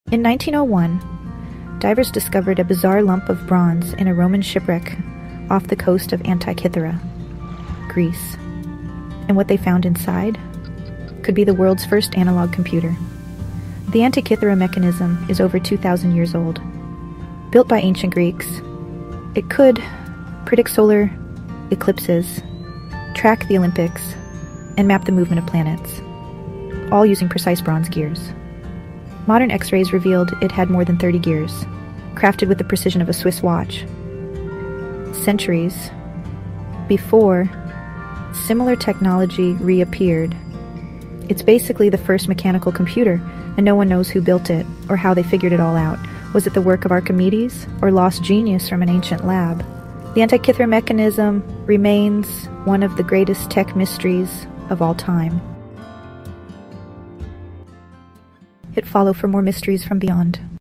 The Mystery of the Antikythera Mechanism – The World’s First Analog Computer
Dive into the secrets of the Antikythera Mechanism, the world’s first analog computer! Discovered in a Roman shipwreck in 1901, this ancient marvel dates back over 2,000 years. Crafted by the ancient Greeks, it could predict solar eclipses and track celestial bodies—all with intricate bronze gears. Modern X-ray analysis unveiled over 30 gears, showcasing engineering brilliance centuries ahead of its time. But the question remains, who created this incredible device? Was it the legendary Archimedes or an unknown genius?

Photo images courtesy of Wikepedia